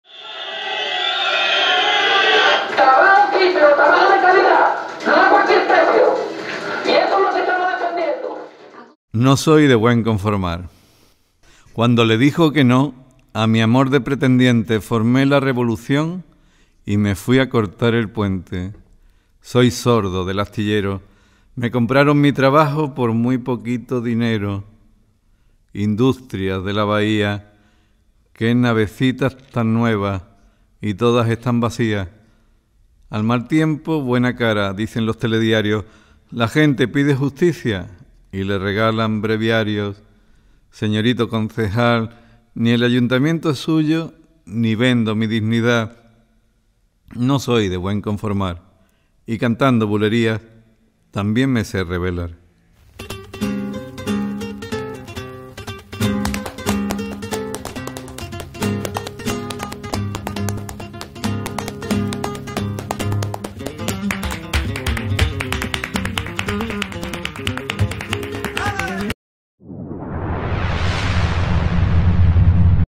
Huelga del Metal en Cadiz !! No soy de buen conformar !! Juan J.Tellez
Huelga en el sector del metal de la provincia de Cádiz en protesta por el bloqueo en la negociación del convenio colectivo que afecta a 10.000 trabajadores. Según los sindicatos, el seguimiento de la huelga en el día de ayer fue del cien por cien en las grandes industrias como Alestis, Navantia y Airbus

Los sindicatos insisten  en que lo importante es la negociación del nuevo convenio con la patronal, evitar los recortes de salario y de derechos laborales

Voz y textto de Juan Jose Tellez ..No soy de buen conformar

Por AdolfoMayo